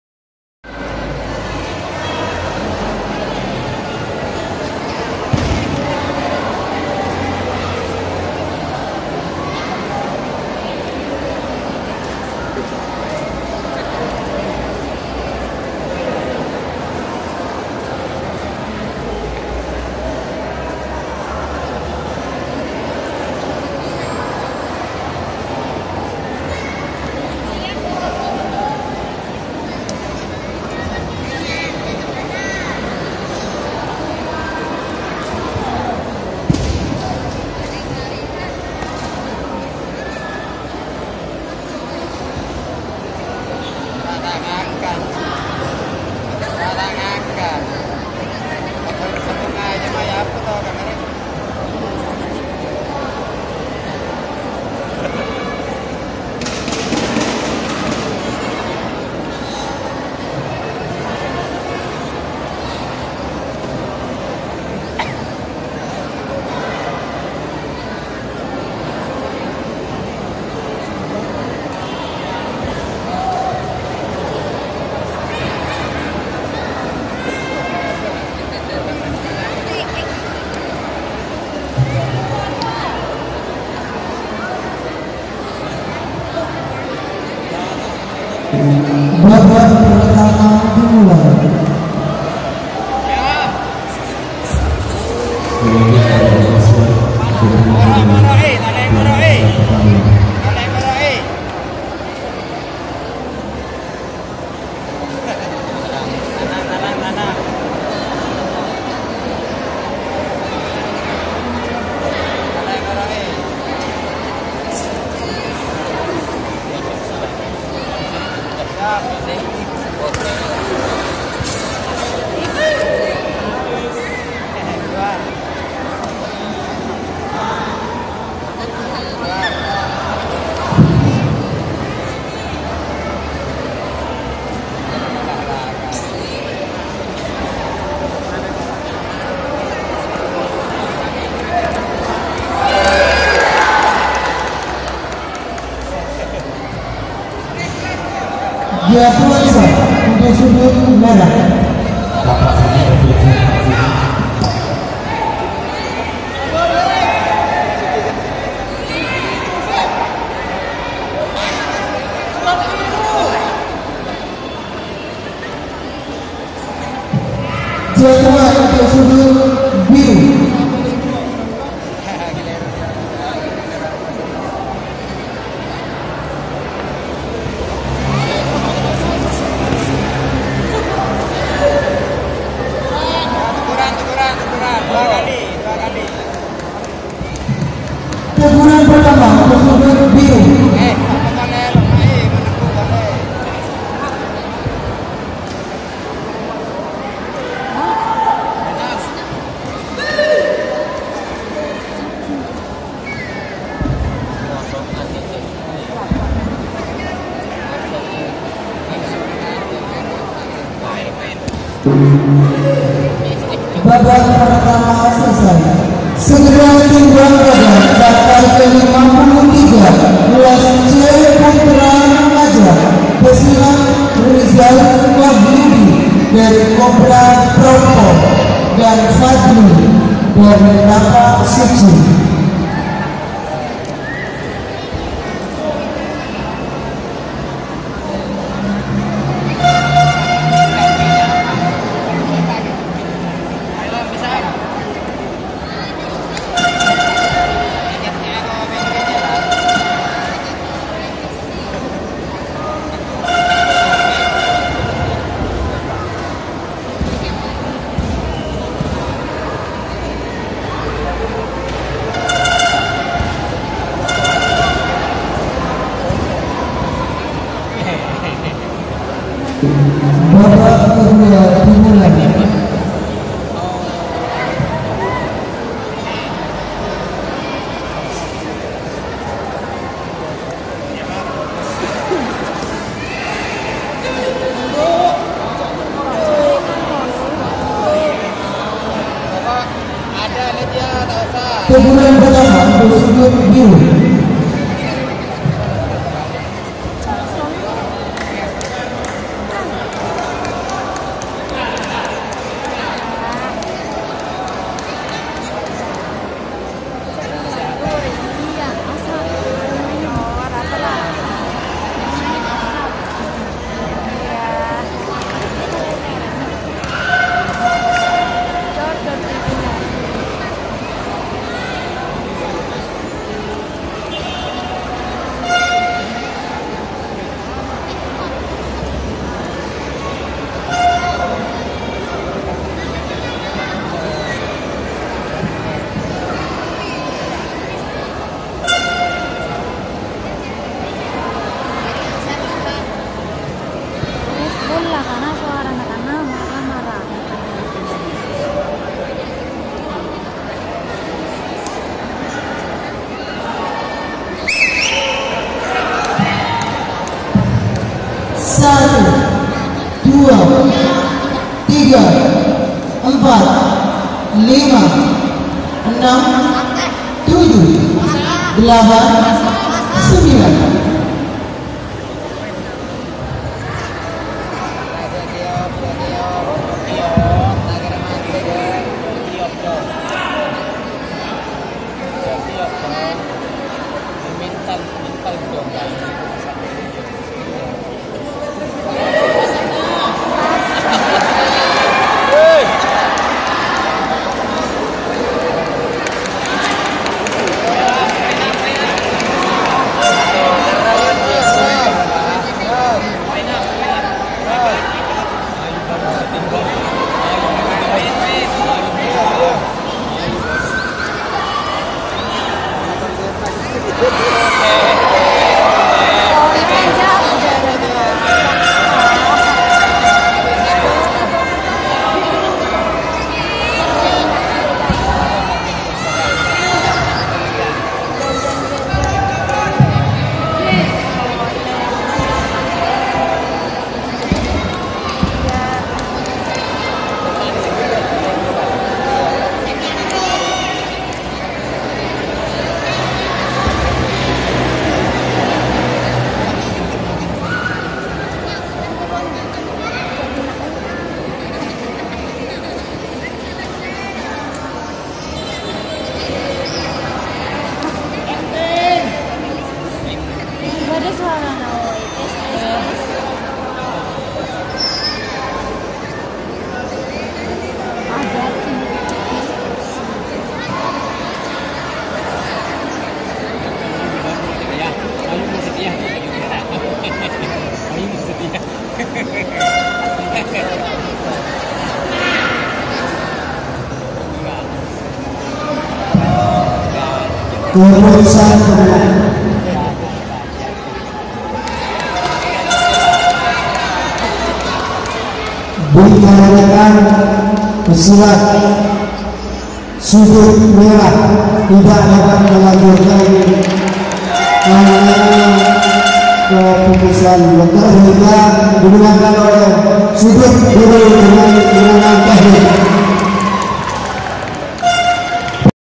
COBRA CUP PERSINAS ASAD GOR NYALARAN PAMEKASAN MALAM
COBRA CUP CHAMPIONSHIP 2019
17 Januari 2019
Kejurkab Pencak SIlat
HARI JADI PS. COBRA KE-42
Bertempat di GOR NYALARAN PAMEKASAN
Kategori Usia Dini, Remaja, dan Pra Remaja